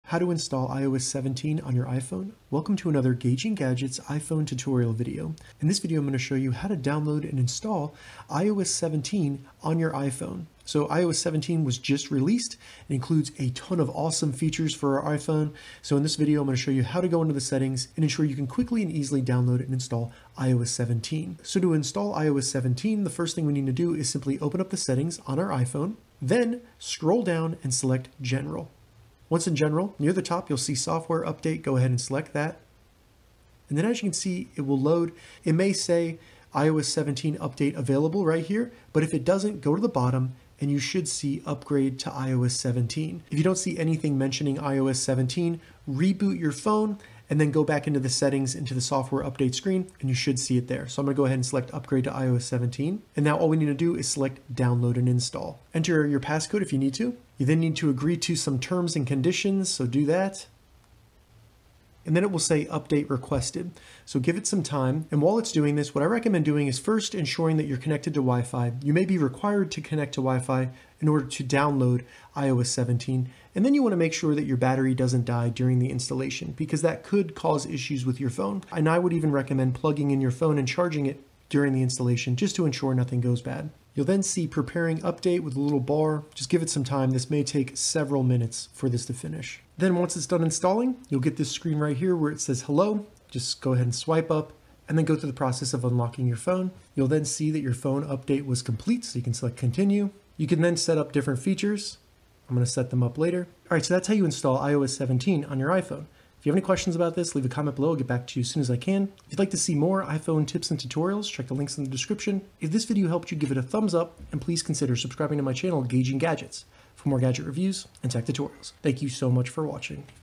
How to Install iOS 17 - Get iOS 17 on iPhone
How to update your iPhone to iOS 17?  In this tutorial, I show you how to simply download the new iOS 17 on your iPhone.  Updating your iPhone to iOS 17 does not require iTunes or a Computer.

Note: this tutorial does require your phone to be connected to Wifi internet to update to iOS 17.

Follow the steps below to download iOS 17 on your iPhone:
1. Open the Settings.
2. Scroll down and select General.
3. Select Software Update.  This will prompt your phone to check for a new update.
4. iOS 17 should appear.  Make sure that your phone is connected to a WiFi network and select Download and Install.
5. If you have a passcode or lock on your phone, you will be prompted to enter it at this point.
6. Agree again.  This will prompt the download to begin.  Please note that the download will take some time and should be started when you aren't planning on using your phone for at least 30 minutes.  Keep your phone charging during the update to ensure that it doesn't die mid-upgrade.
7.  Once your phone is done downloading, select Install Now.  This should restart your iPhone and show a progress bar for the duration of the installation.
8.  Once installation is complete, you will be prompted to press the home button to finish setting up iOS 17.  Press the Home button to continue.  Enter your passcode (if set up).
9. You should see Update Completed.  Select Continue.  If you would like to setup Apple Wallet, you can do so with the next prompt.  You should see an option for the Dark Mode feature.  Select between Light and Dark Mode.  The last prompt should say Welcome to iPhone with the option to Get Started.
10. Under Software Updates, you should see iOS 17.0.  You have now completed the new install of iOS 17 on your iPhone!

iOS 16 is compatible with the following iPhones:
 • iPhone XS
 • iPhone XS Max
 • iPhone XR
 • iPhone 11
 • iPhone 12
 • iPhone 13
 • iPhone SE (second generation and later)
 • iPhone 14